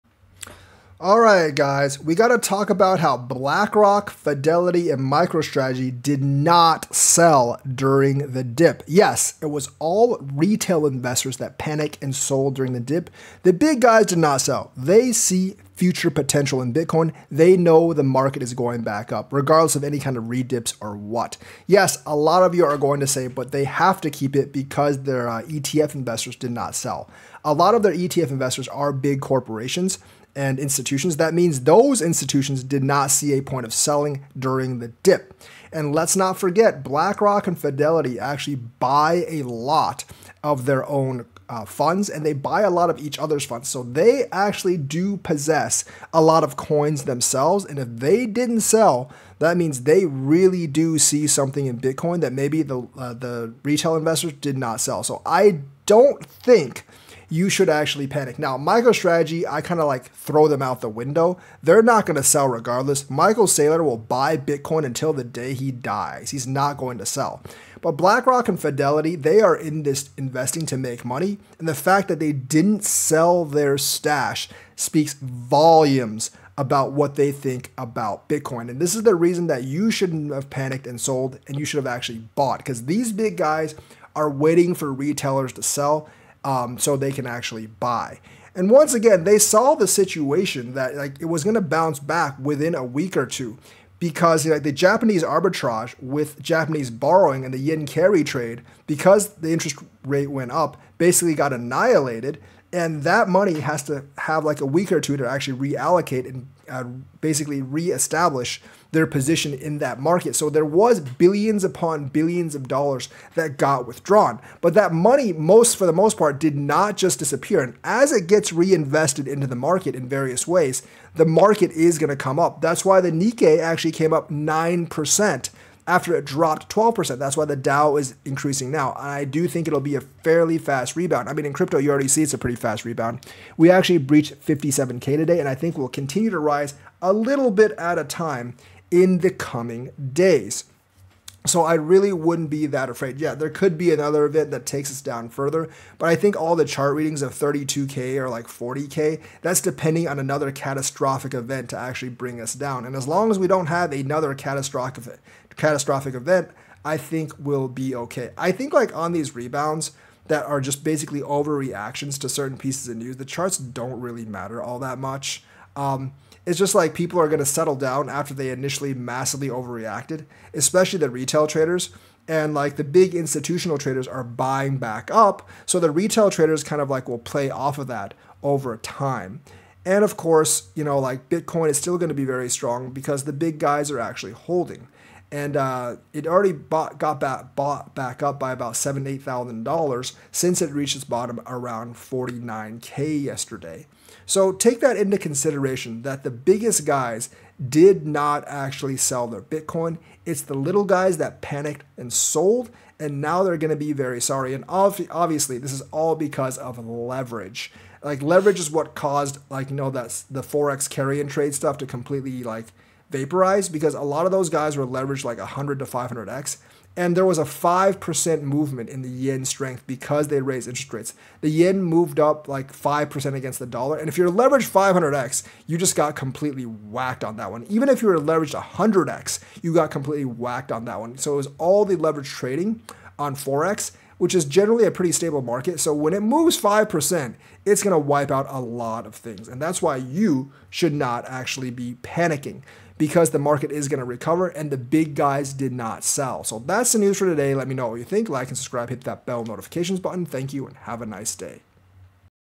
Blackrock, Whales did NOT SELL Bitcoin during the Dip. Its a retail trap!!
Black and the other whales DID NOT SELL during the Dip. Its a retail trap.

Bitcoin:
3Ajg5RxG6a4BjCQXxabPsPCepv88126DXd

ADA:

addr1vxg6juuzlf8ljq982ew97mtlmuf4ef05gyz4t700dd32pyqj383da

Solana:
UWyRgseiuQ1ZQ6ShVbAwTn46dKGPkHeSjwqaYpE1oQh

Jasmy:
0x264a1864F5809E67F495B3eB2Db2d17981a8fB63